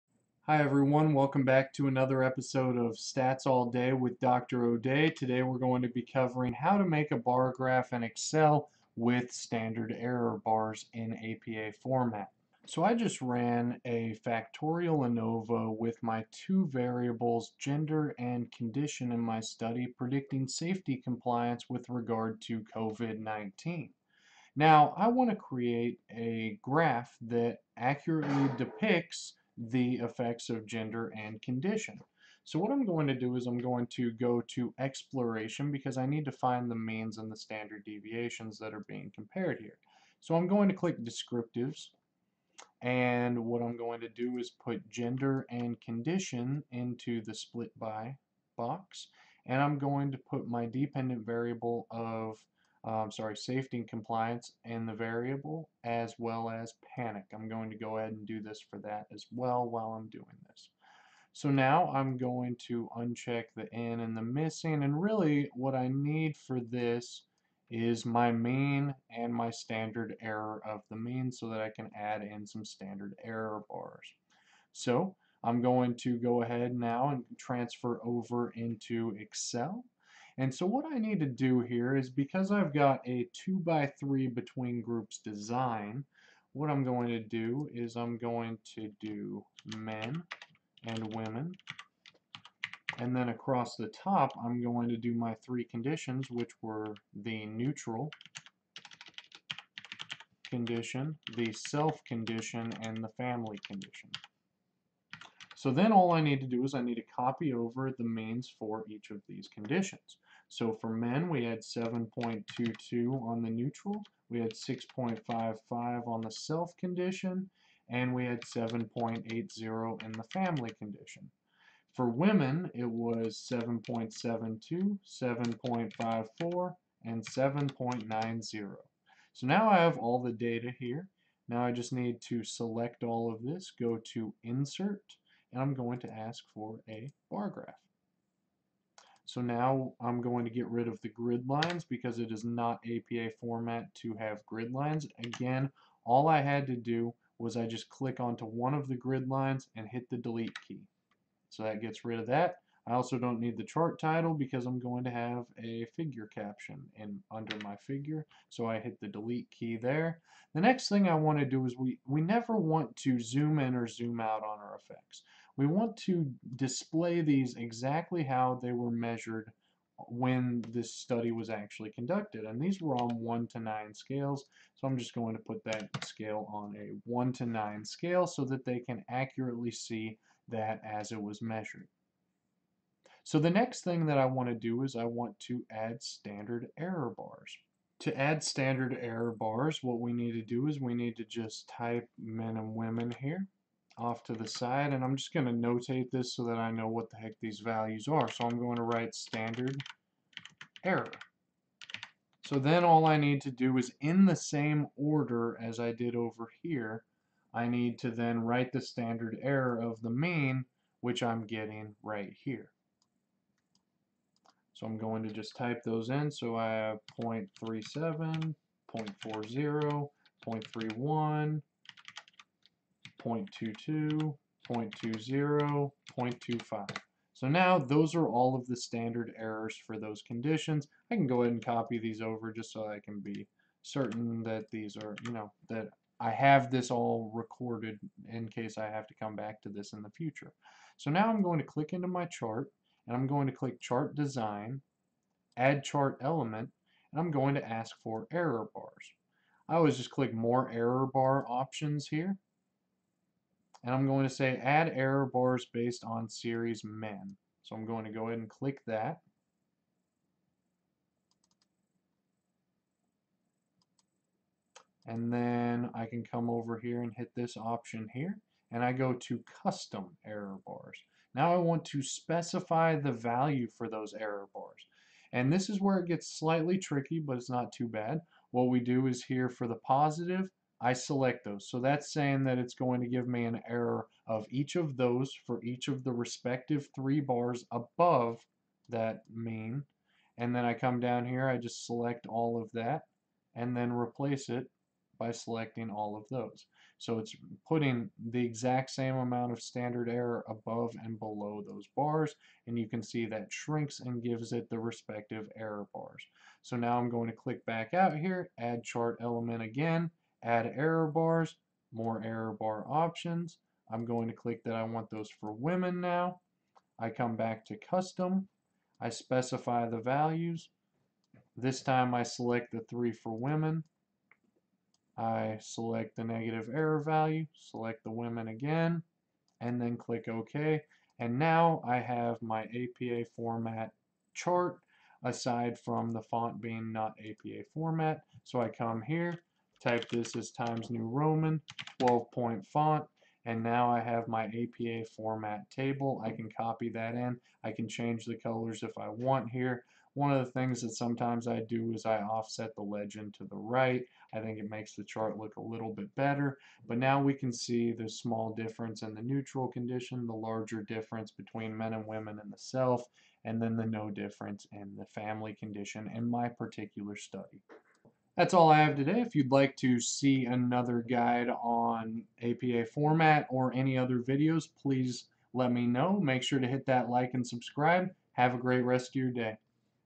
APA format : Bar graph in Excel
In this video I describe how to make a bar graph with error bars in Excel. Please let me know if you have any questions!